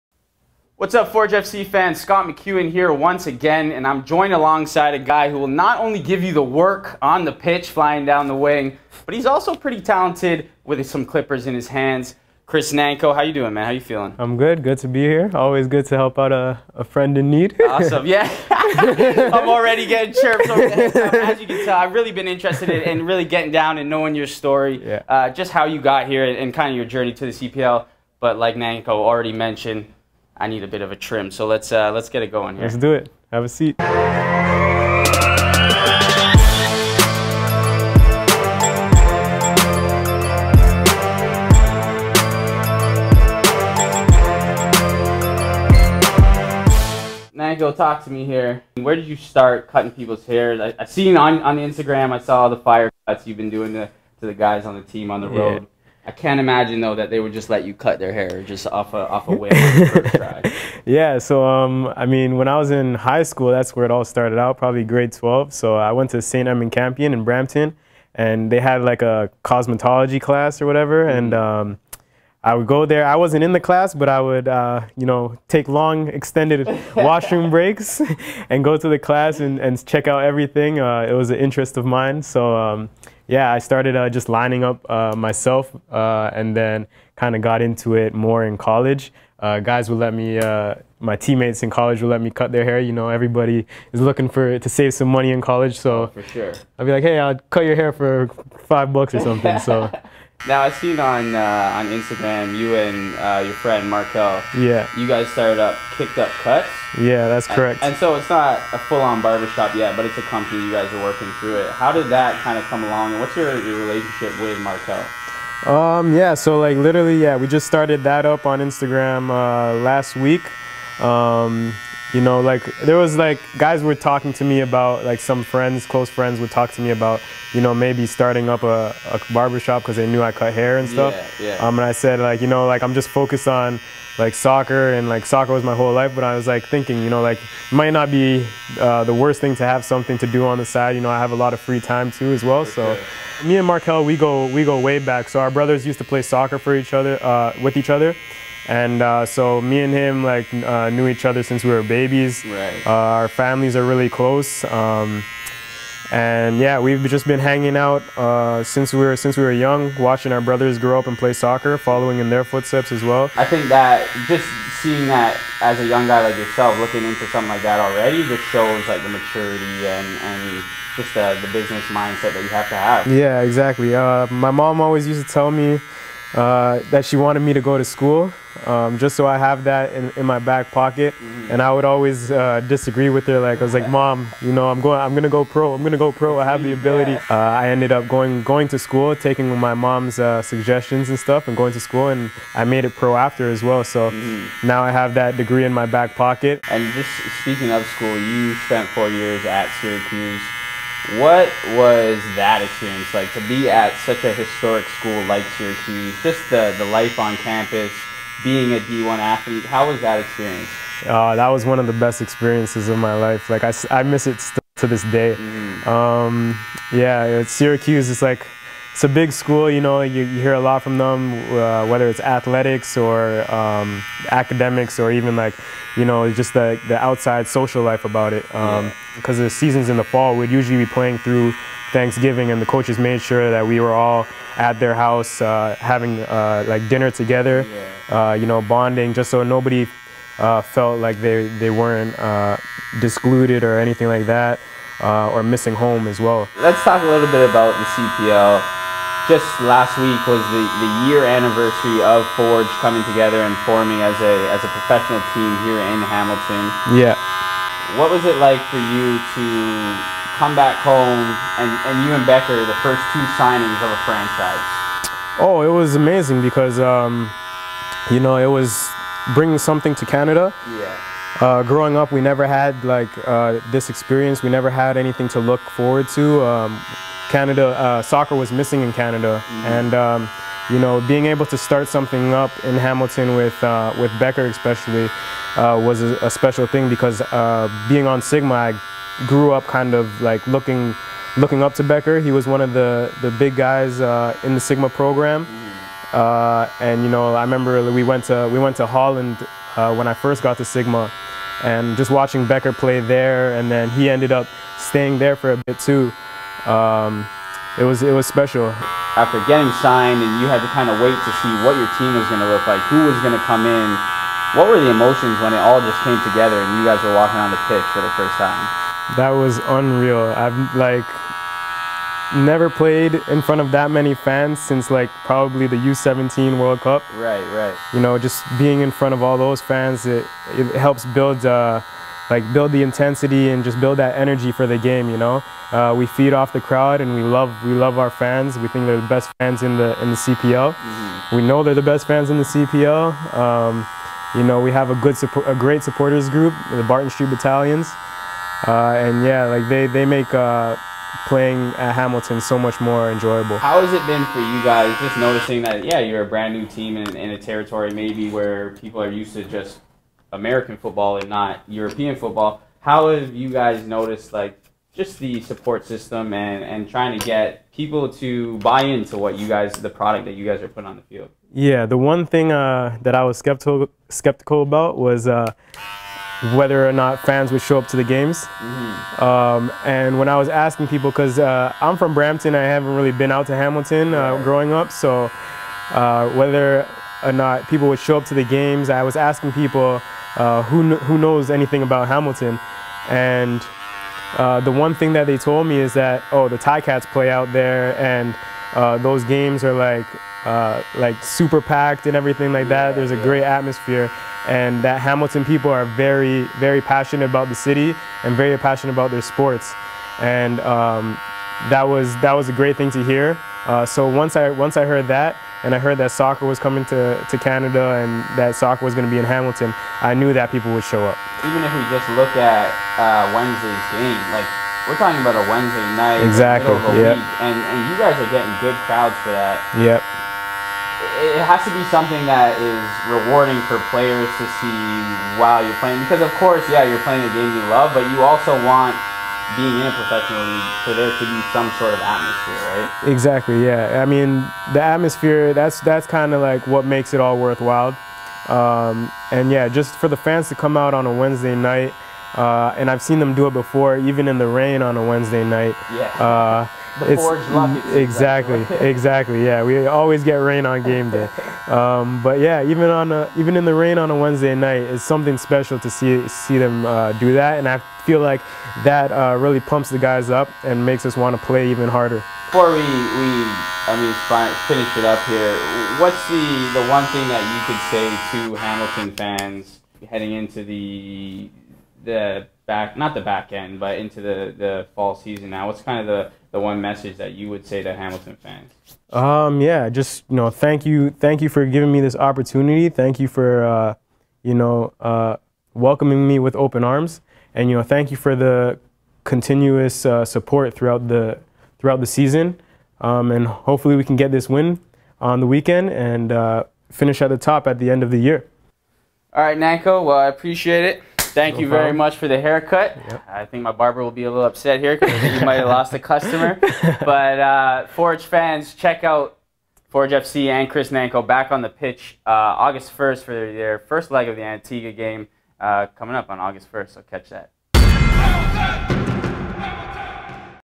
Fresh Cuts with Chris Nanco
Scott McEwen sits down with Forge FC's Chris Nanco to get a cut and talk starting up his own company, CPL, Syracuse University and more.